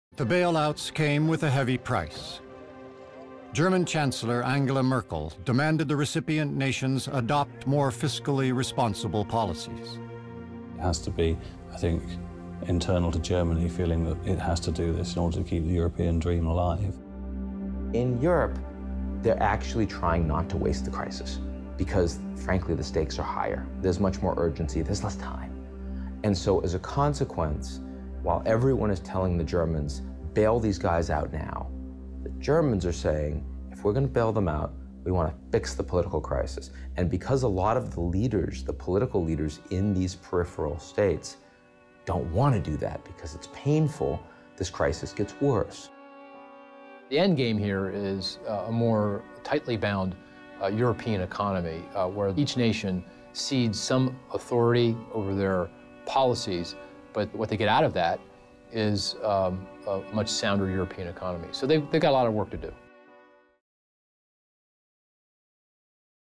Keeping the European Dream Alive
The bailouts to save the crumbling European economies came at a heavy price. Yet as Greece continues to tighten its belt to stay within the eurozone, France's Hollande has stated, "Greece needs to be supported by the whole of Europe" as it undergoes this time of trial. Find out more in "Imperfect Union: The Eurozone in Crisis."